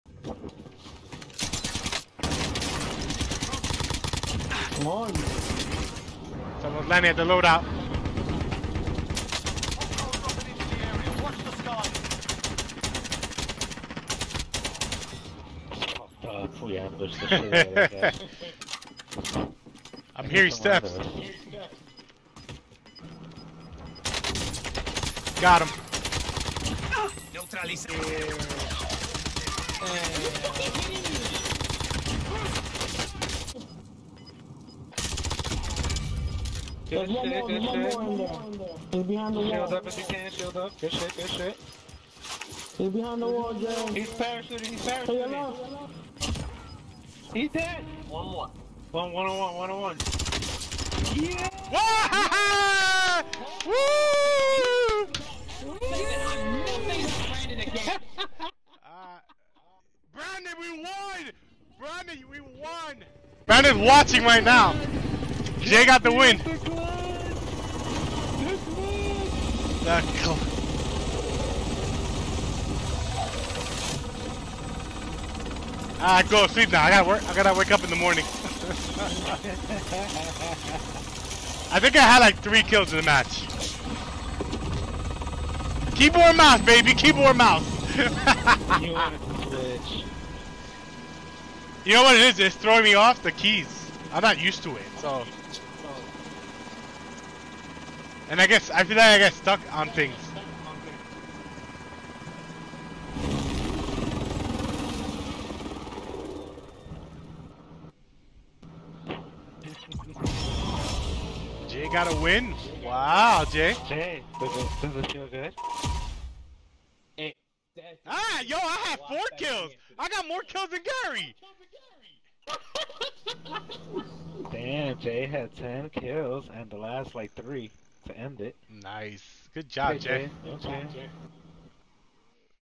WGZGaming: October 3, 2021 Victory Highlight - Jay strikes Back.
If you saw Jay’s rage moment, he was clutch tonight. Got the team win. Let’s go Jay.